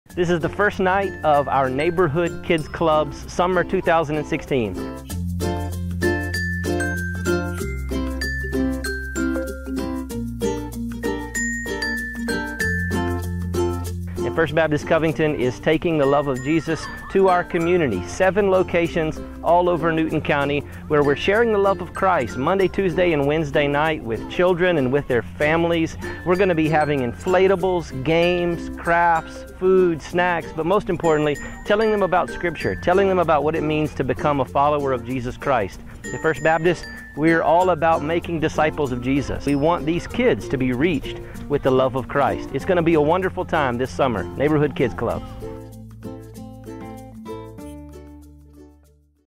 Neighborhood Kids Clubs - VBS 2016
First Baptist Covington hosted VBS in neighborhoods this year, and here is a look at some of the things they did.

to see upcoming events.